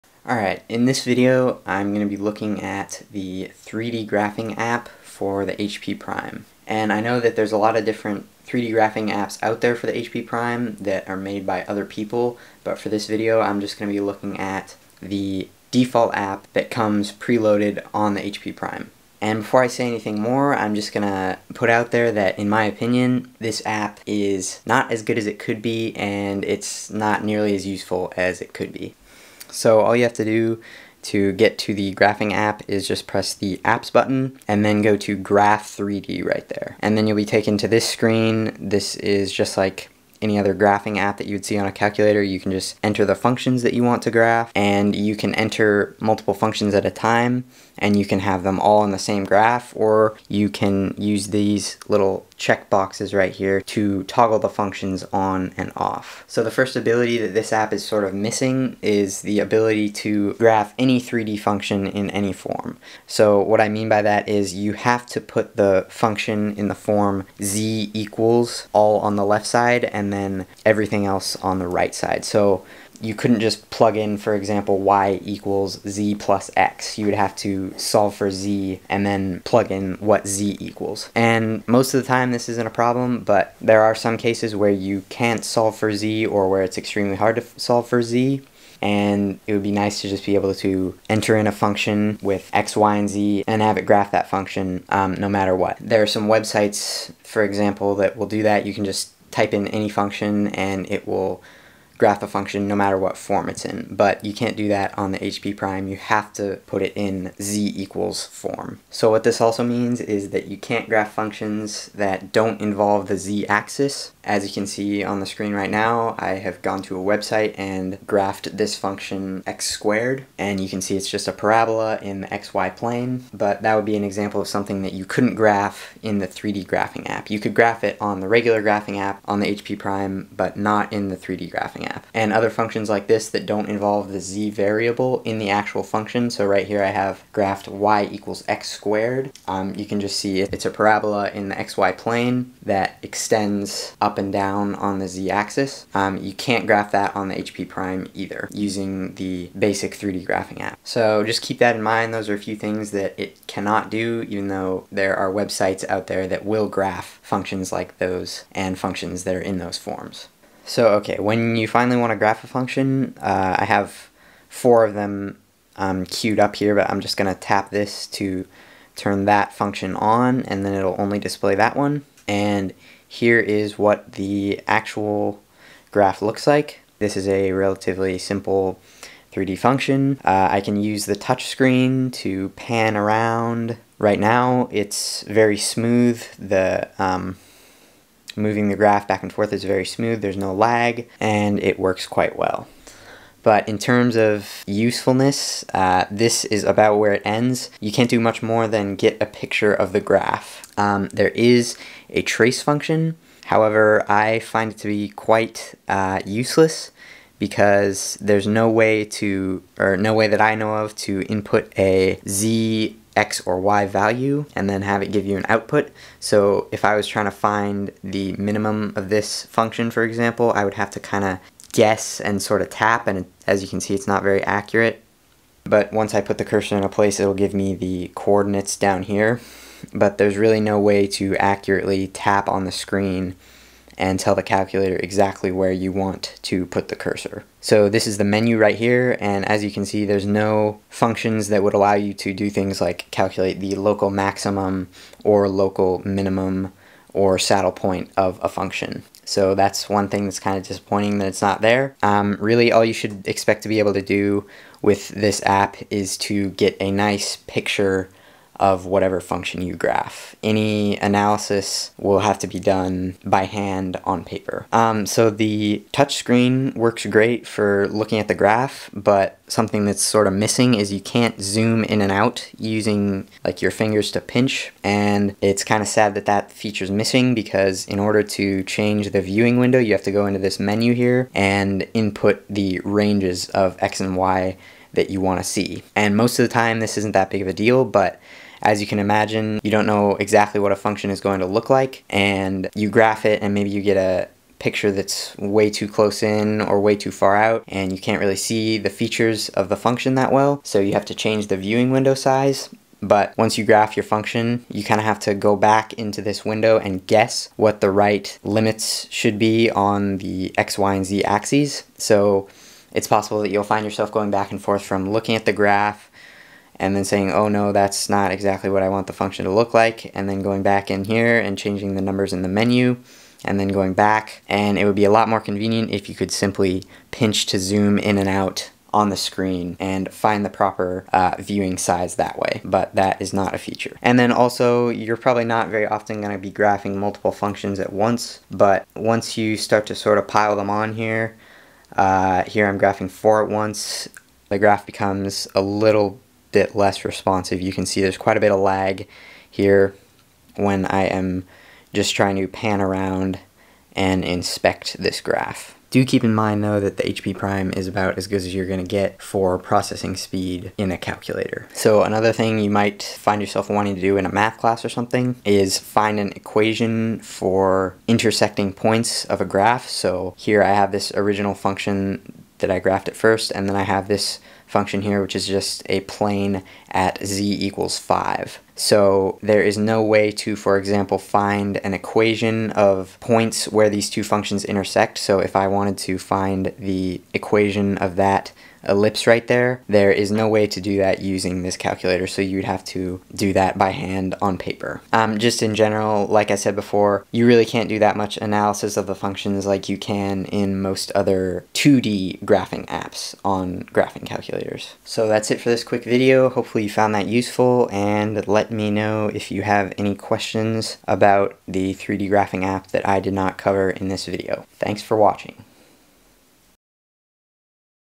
3D Graphing On The HP Prime Graphing Calculator
The HP Prime has a built in 3D graphing app that allows the user to display the graph of a multivariable 3D function. The app is somewhat limited in features and cannot do much in terms of analyzing the graph after it is produced.
I was not very impressed by this 3D graphing app but it is comparable to a lot of other graphing calculators out there.
Other individuals have created (coded) their own 3D graphing apps that are better than the default app that comes with the calculator. Specifically, one of the best ones, was created by Han on the HP Museum.